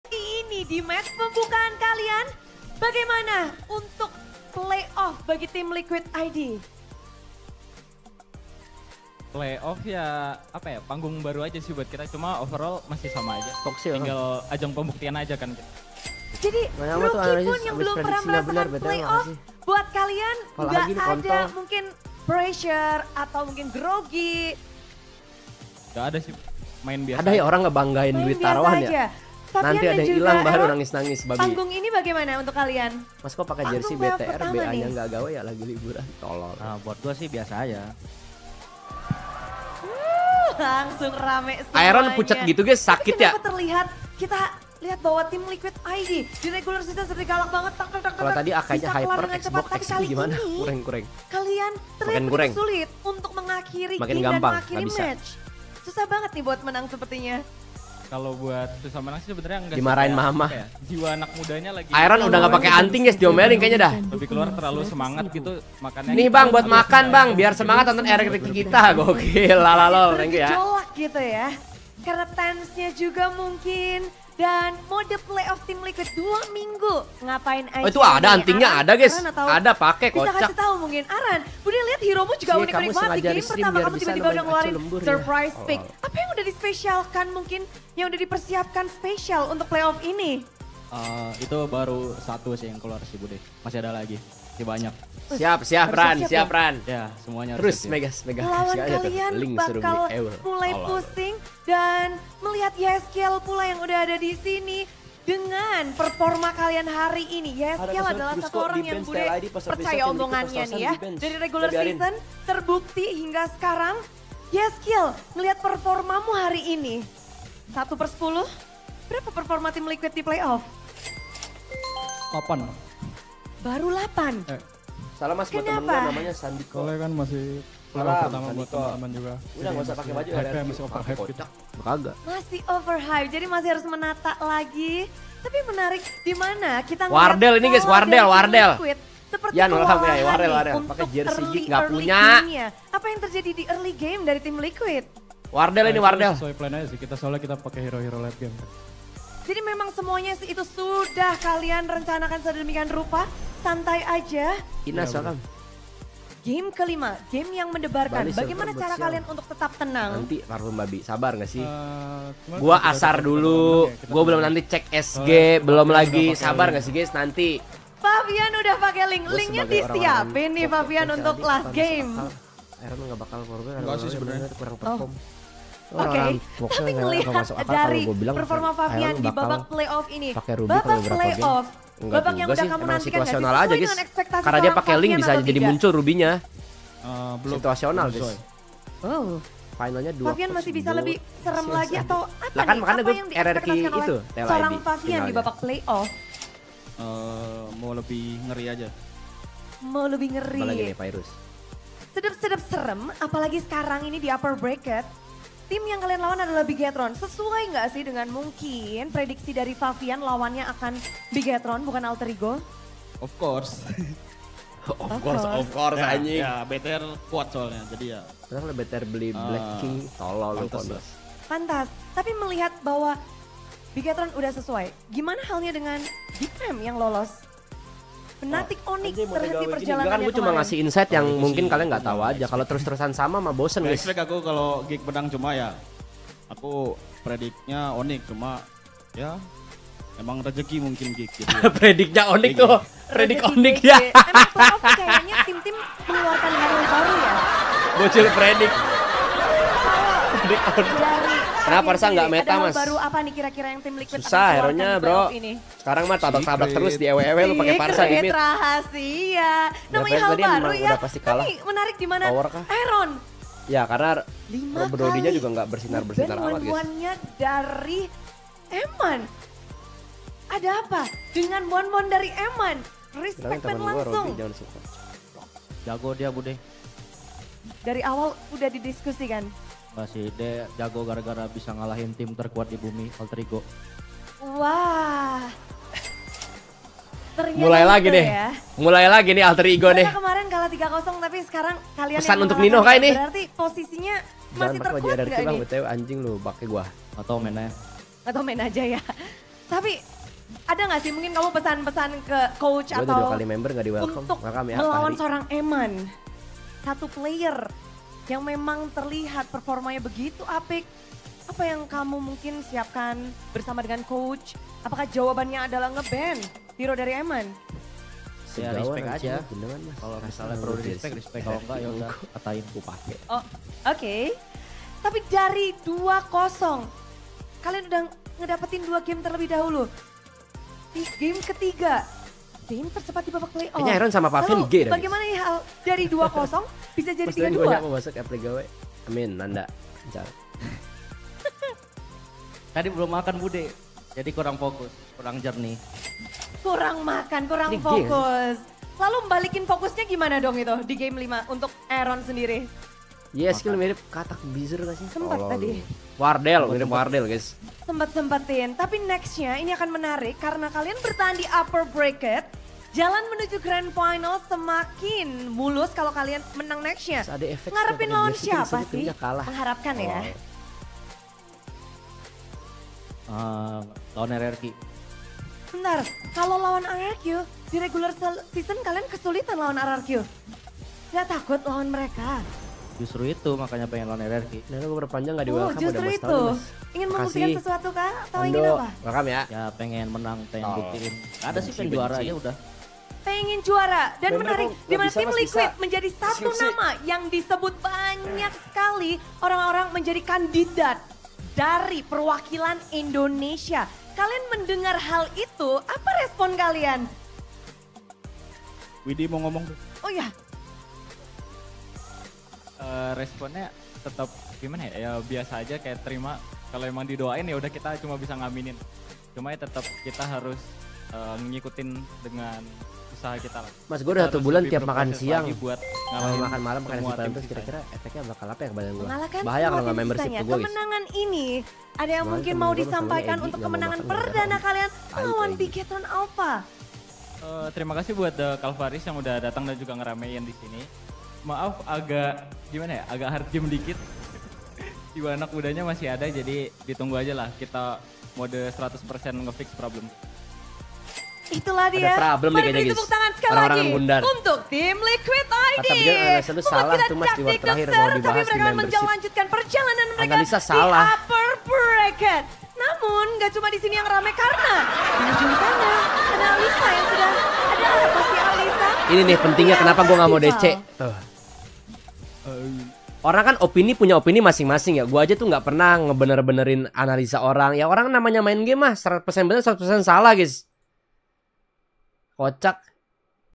Interview Kemenangan LTID vs BTR - Aeron Ga Maksimal Sakit Kah ? Reaksi Mas Ade
Hello Channel Welcome To My Guys.